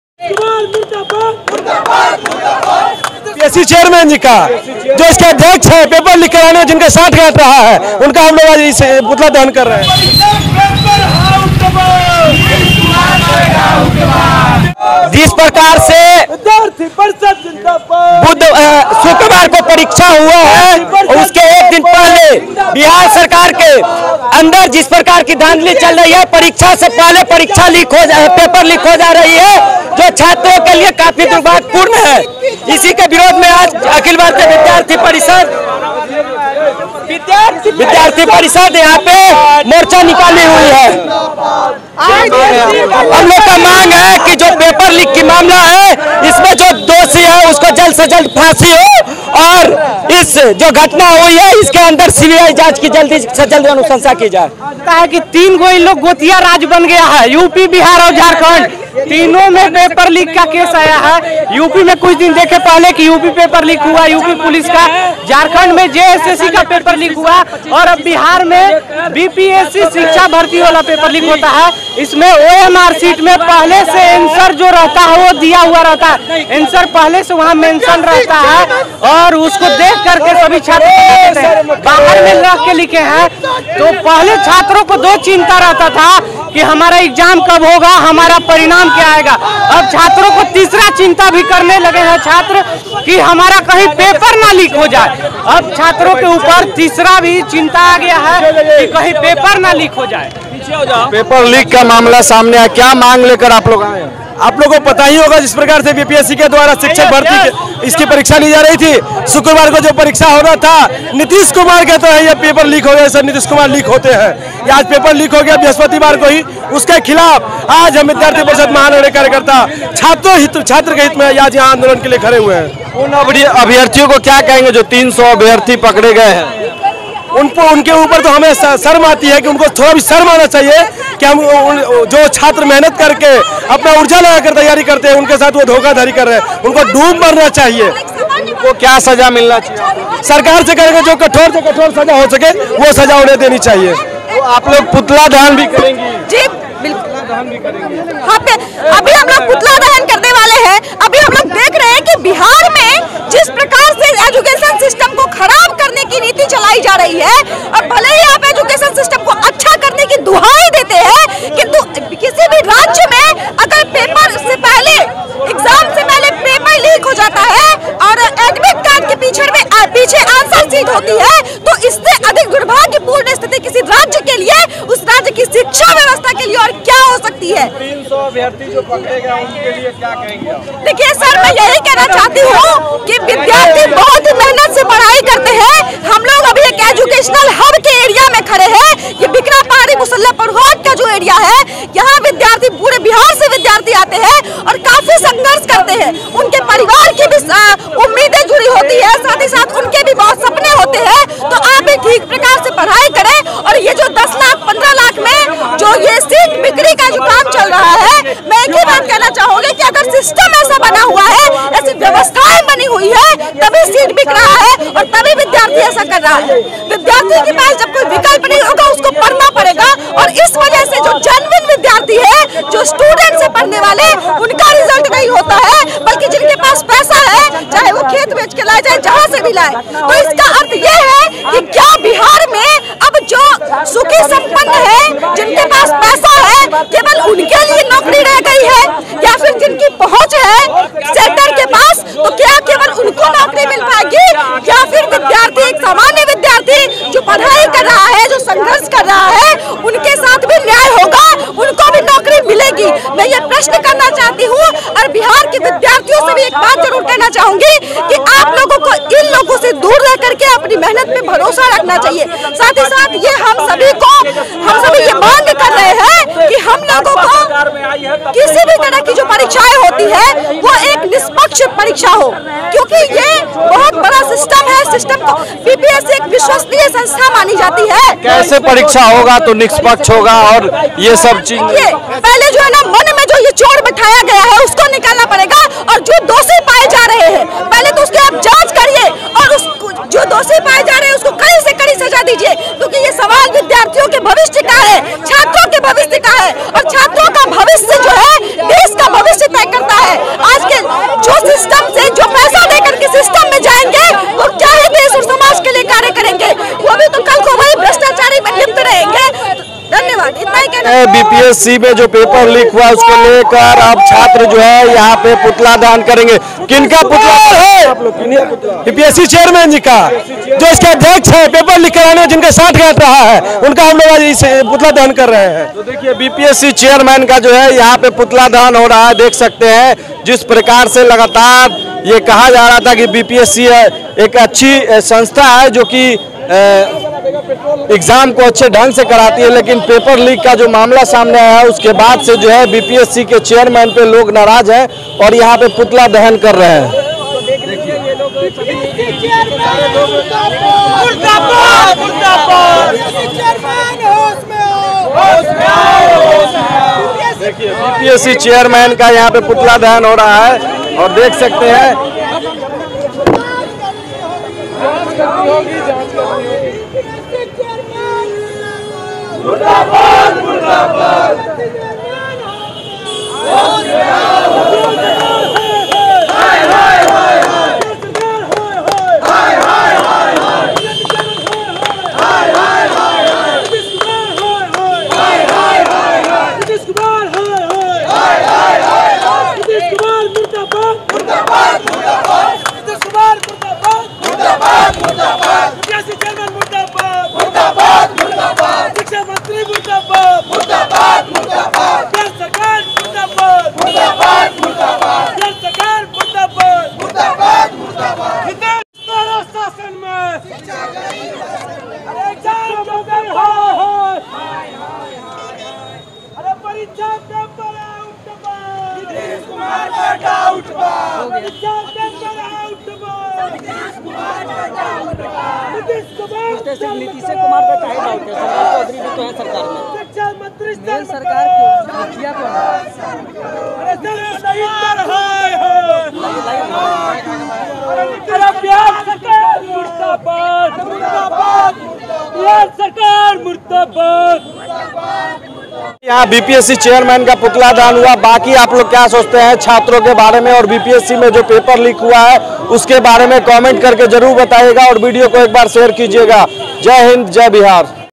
BPSC TRE 3.0 पेपर लीक पे ABVP छात्रों ने अध्यक्ष का ज*ला*या पुतला, नीतीश कुमार भी होंगे हैरान 😭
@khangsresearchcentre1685